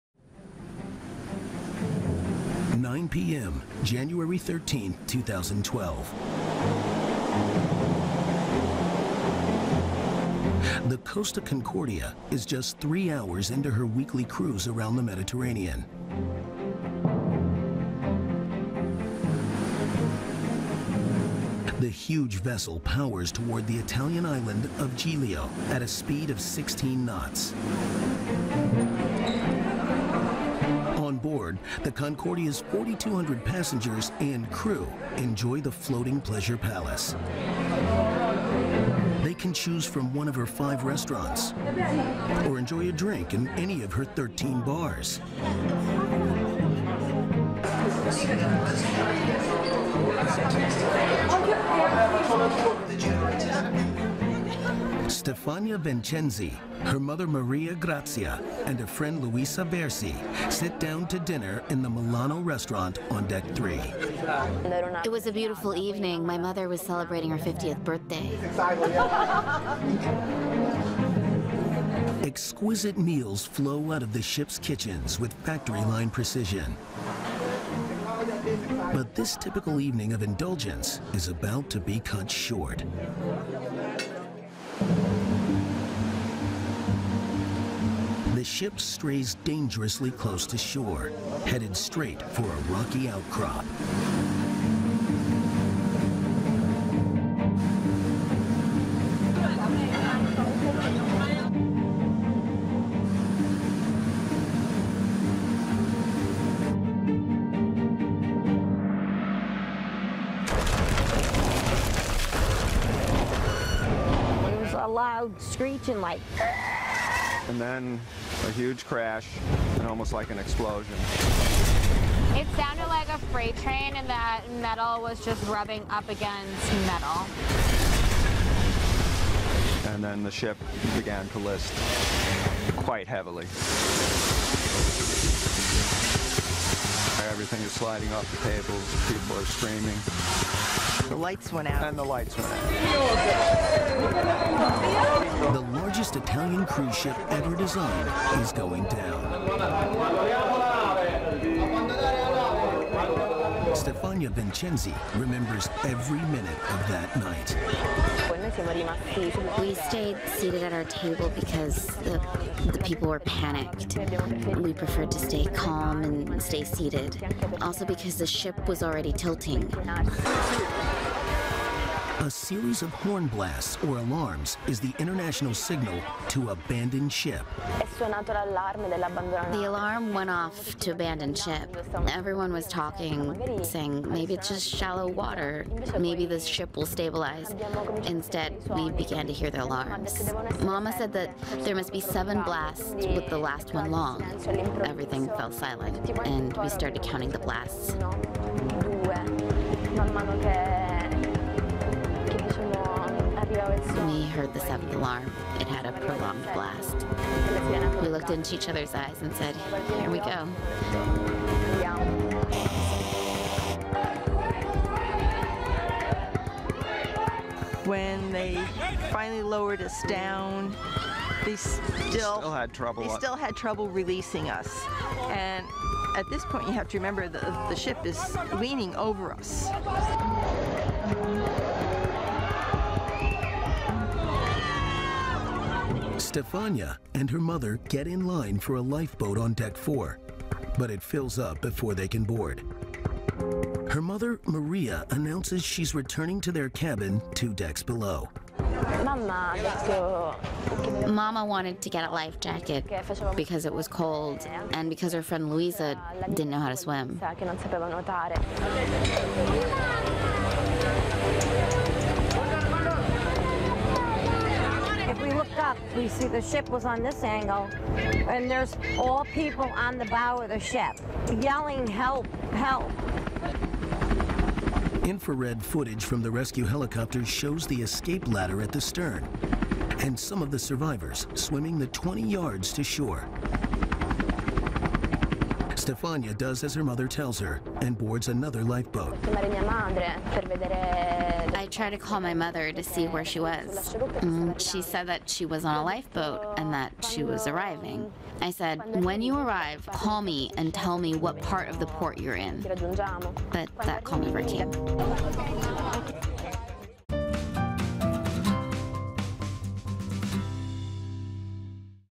Costa Concordia: Chaotic Footage from Inside the Crash
Three hours into her weekly cruise around the Mediterranean, Costa Concordia crashes into an outcrop of rocks, sending passengers into a frenzied panic.